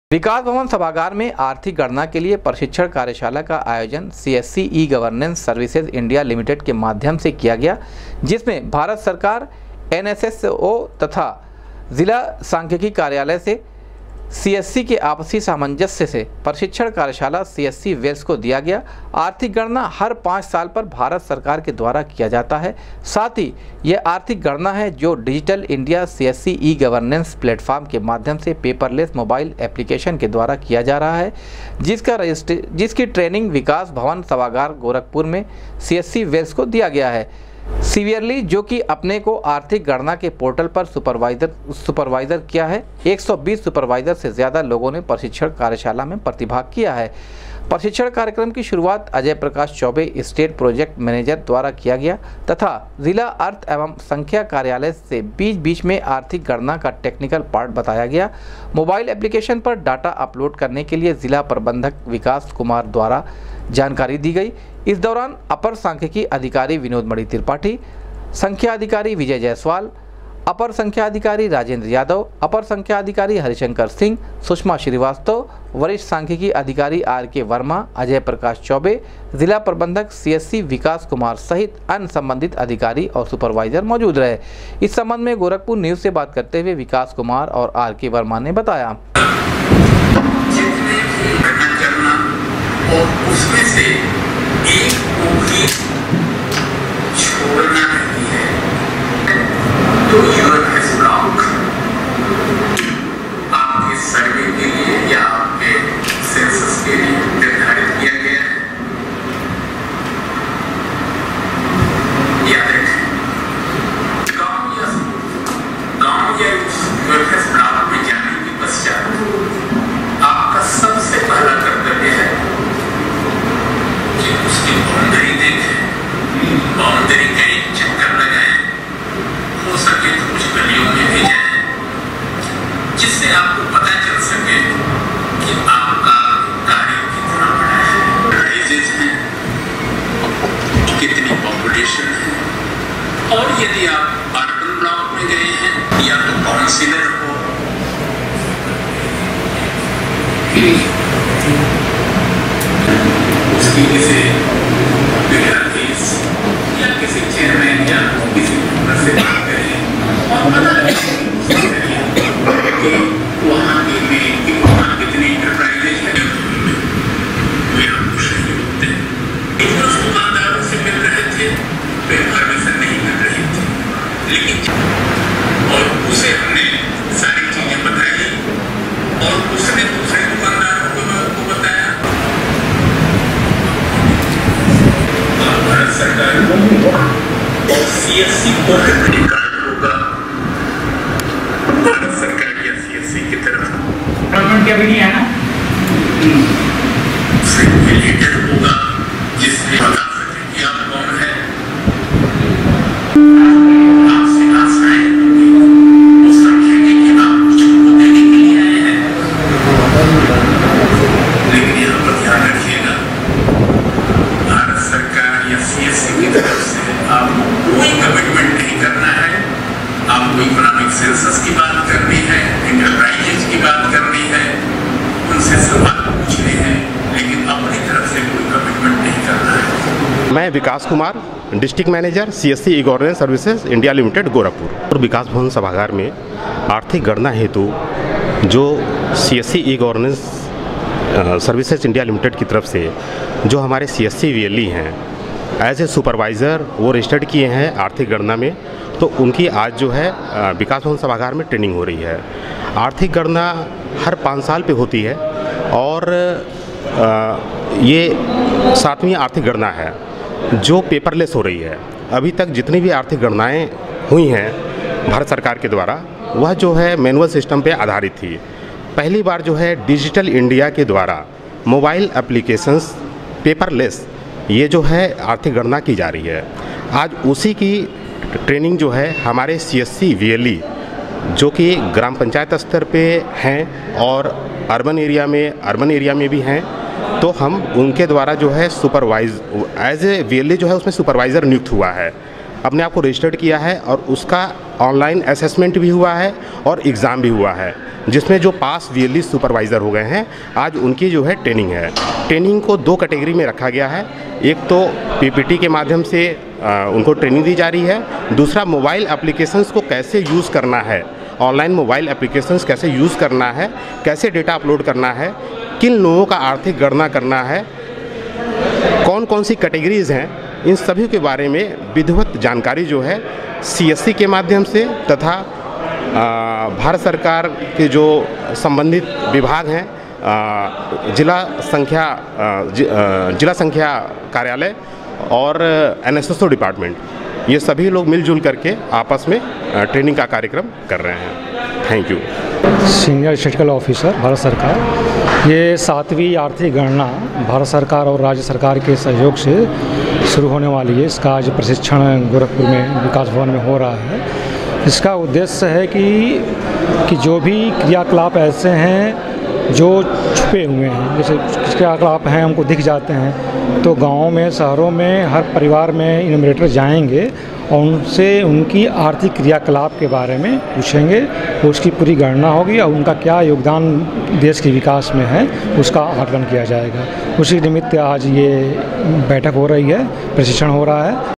आर्थिक गणना के लिए विकास भवन में आयोजित हुआ कार्यशाला, 120 सुपरवाइजर से ज्यादा
आर्थिक गणना के लिए विकास भवन में आयोजित हुआ कार्यशाला, 120 सुपरवाइजर से ज्यादा लोगों ने प्रशिक्षण कार्यशाला में किया प्रतिभाग,दी गयी पेपर लेस मोबाइल एप्लीकेशन गणना की जानकारी ।